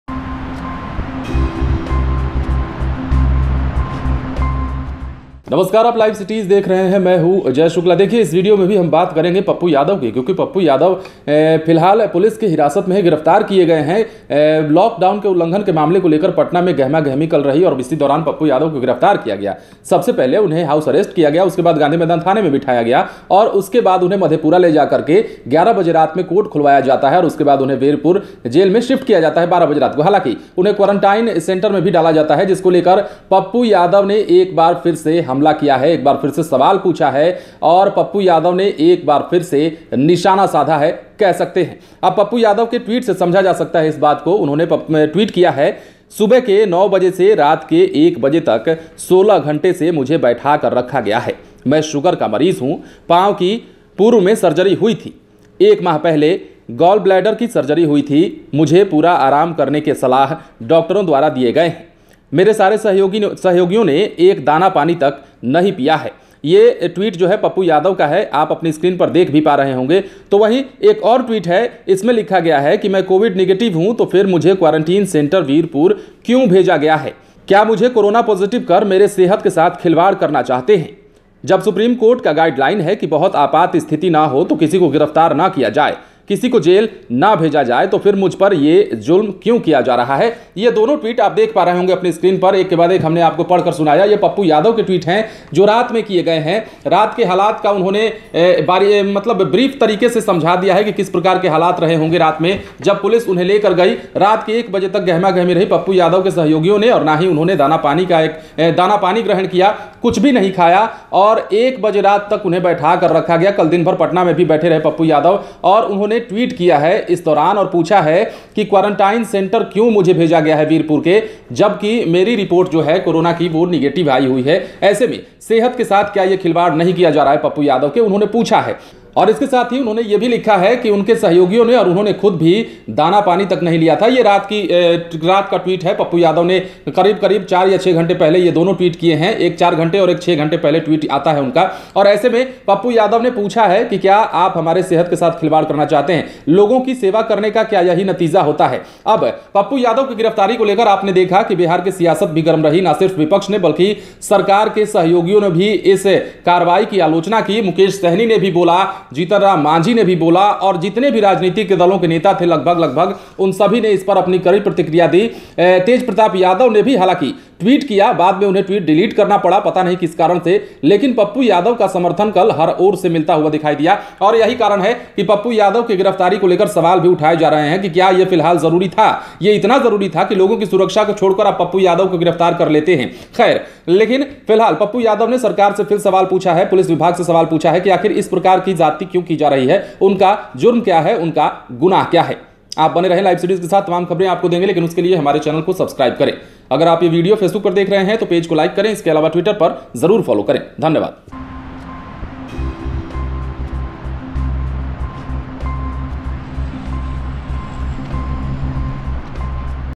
Pappu Yadav फिर दहाड़े, सरकार से पूछ रहे हैं – क्या करने का इरादा है, मेरा जुर्म क्या है ?
Pappu Yadav फिर दहाड़े, सरकार से पूछ रहे हैं– क्या करने का इरादा है, मेरा जुर्म क्या है ? LiveCities |

PappuYadav